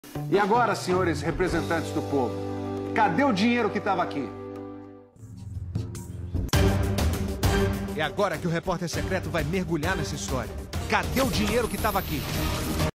CAMARÁ MUNICIPAL DE PEREIRA BARRETO
A POPULAÇÃO DE PEREIRA BARRETO QUER SABER EM 2017 O EX PRESIDENTE DA CAMARÁ MUNICIPAL DEVOLVEU MAIS DE UM MILHÃO JÁ O EX PRESIDENTE DA CAMARÁ CLEBER MARIANI EM 2018 NÃO DEVOLVEU NEM 15 MIL REAIS ONDE FOI PARAR O DINHEIRO SENHOR REPRESENTANTE DO POVO
VOU BATER UM OFICIO PARA A CÂMARA MUNICIPAL SOLICITANDO ESTAS INFORMAÇÕES NÃO ESTOU CRITICANDO O SEU TRABALHO EU APENAS JUNTAMENTE COM A POPULAÇÃO QUER SABER ONDE FOI GASTO O DINHEIRO